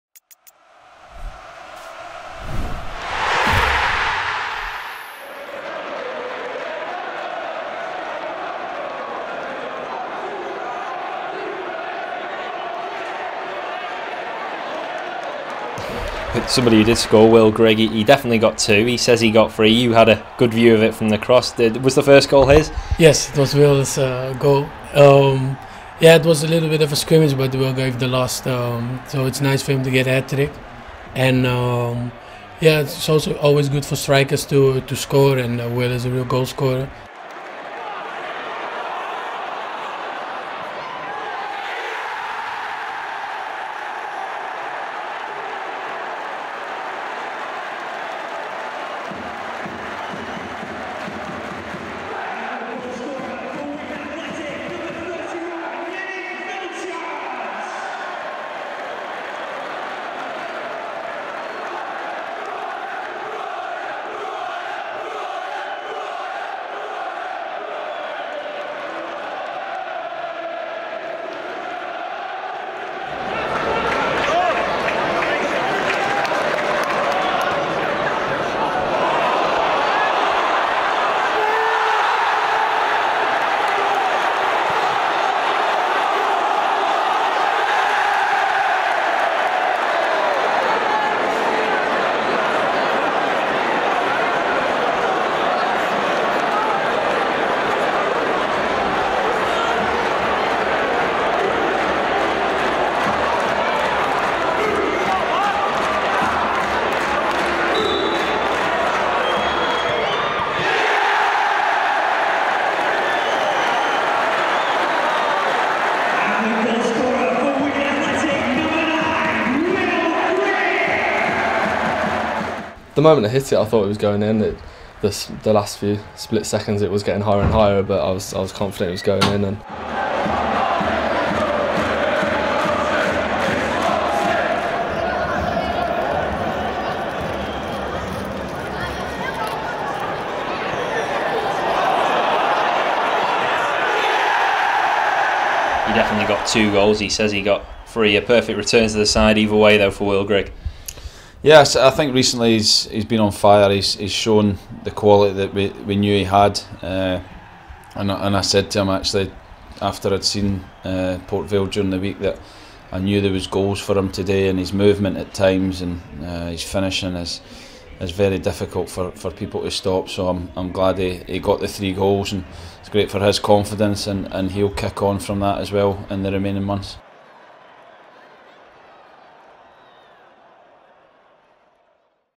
MULTI CAMERA HIGHLIGHTS: Will Grigg hat-trick v Port Vale?
A closer look at Will Grigg's hat-trick against Port Vale in Sky Bet League One.